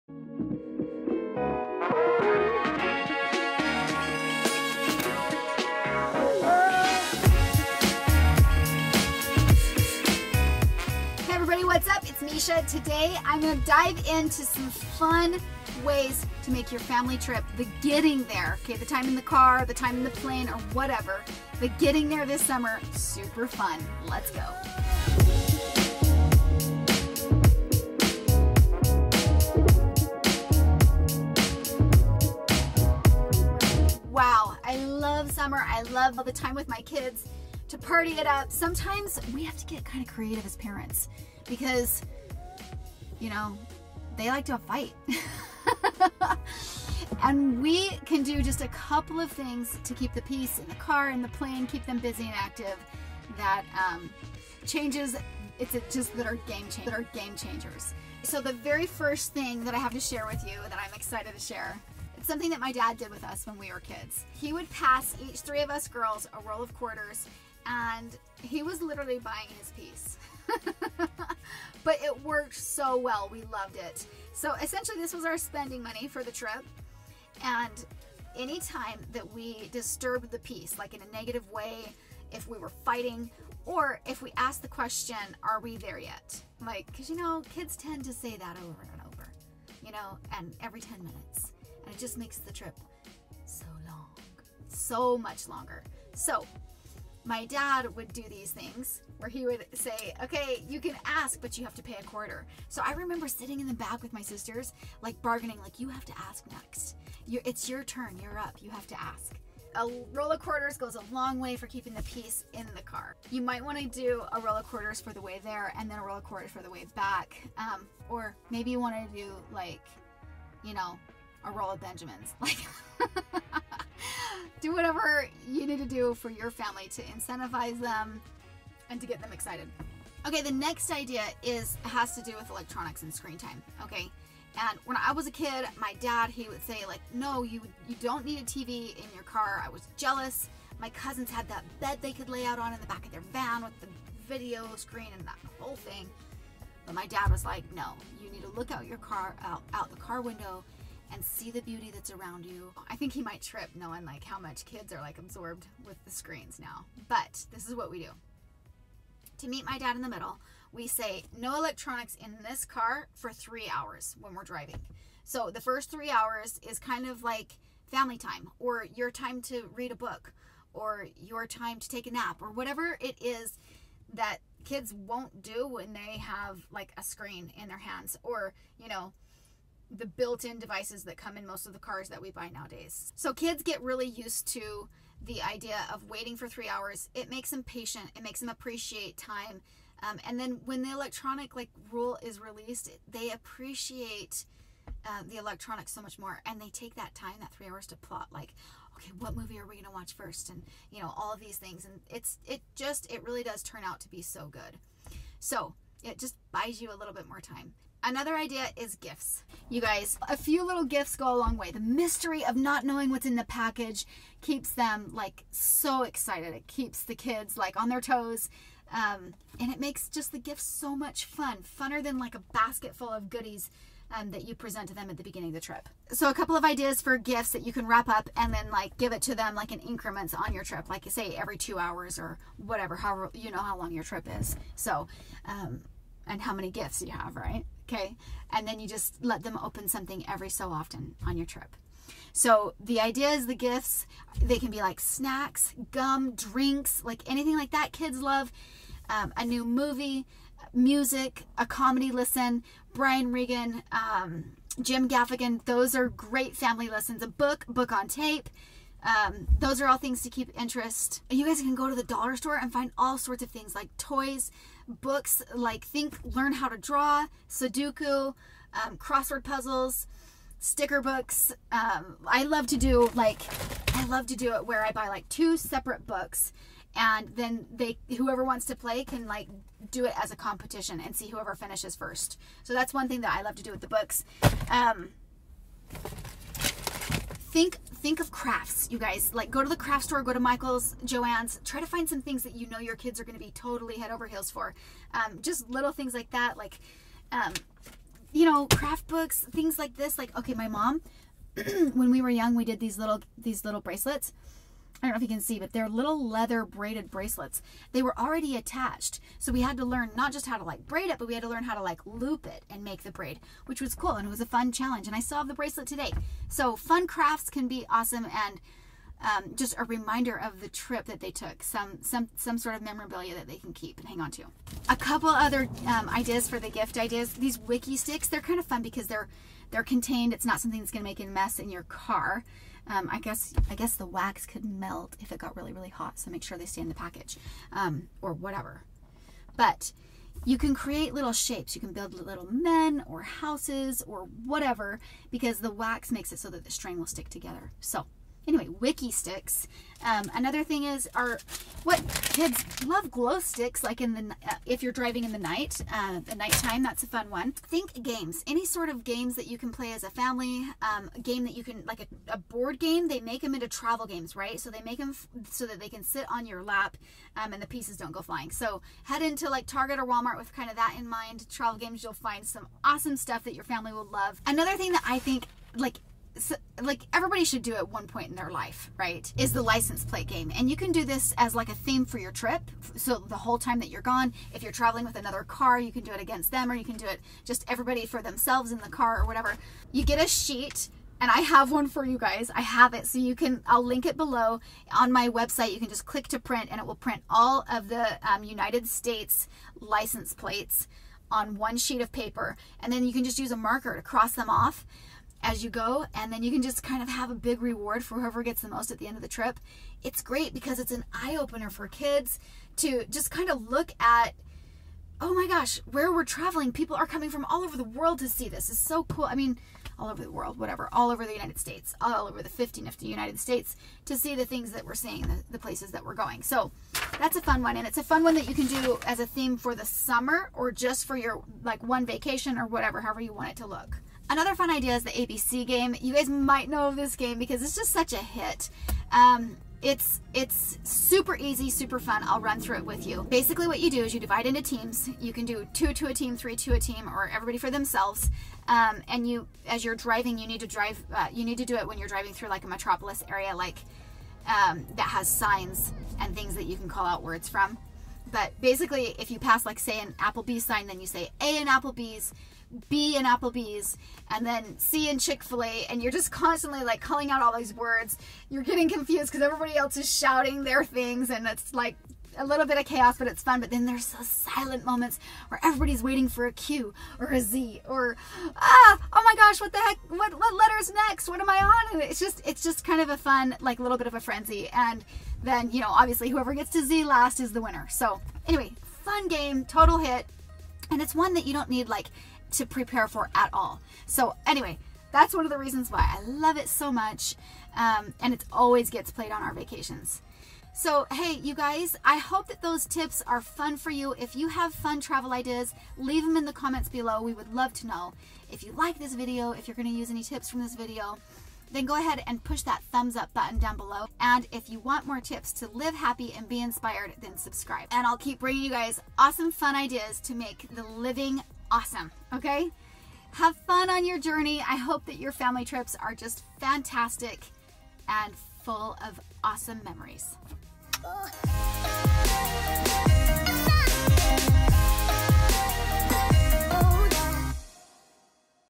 🚗 Road Trippin'! Fun ideas for for traveling with kids!✈️
Make your travel time awesome! Just a little preparation buys you lots of not only peace but fun memories. Have a blast on your vacation!
👇🏼Lots of links below to help you out! 👇🏼

1 - A roll of quarters goes a long way
2 - Three hour electronic rule
3 - Gifts!
4 - The license plate game
5 - The ABC game

Fun gift ideas
Wiki stix
Scratch art
Table topics
Learn to draw
Sticker book